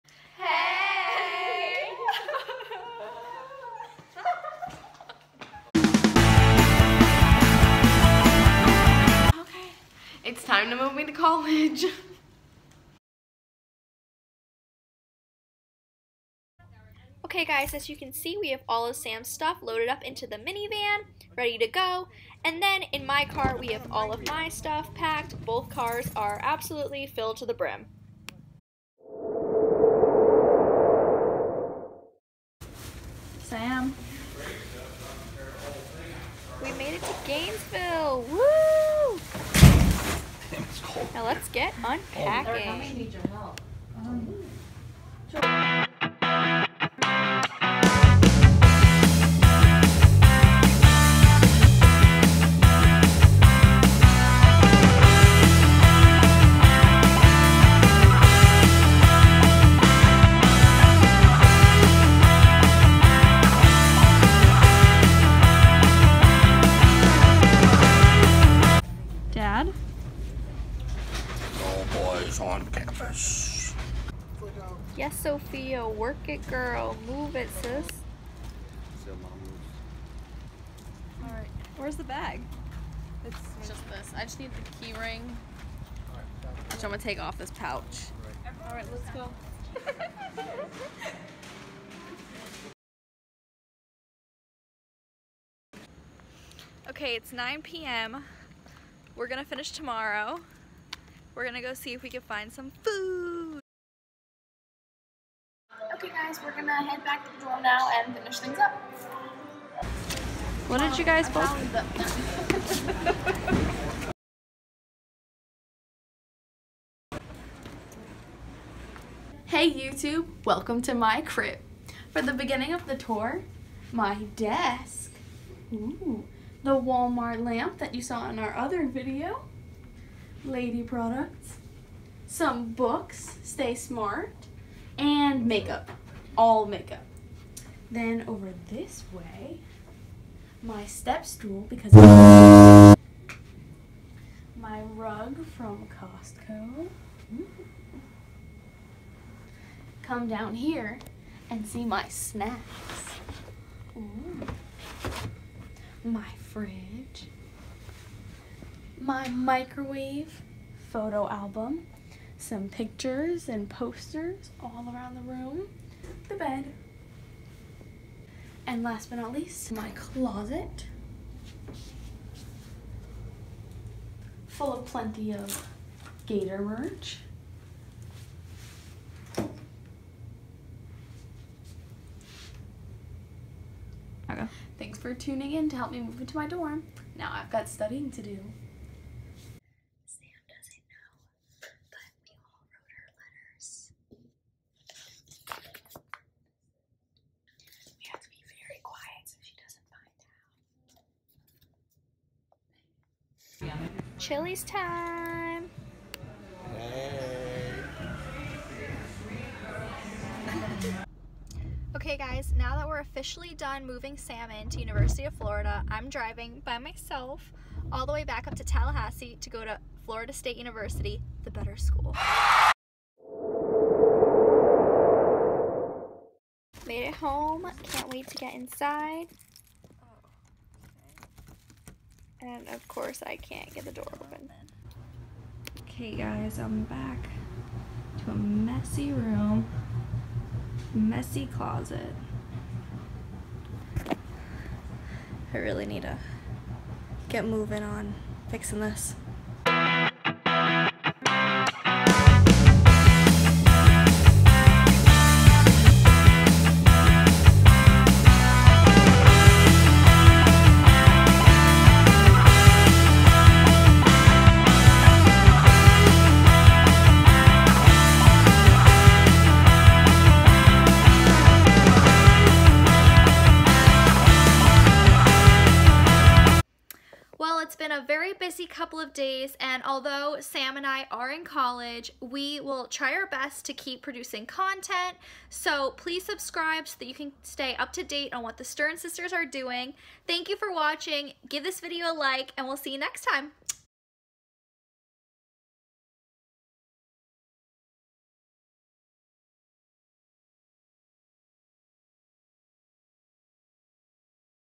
♡ COLLEGE MOVE IN DAY 2019 ♡ / FSU & UF
It was college move in day for 2 of the 3 Stern Sisters! This trip was bitter sweet and we are very thankful for everyone who has been a part of our journey!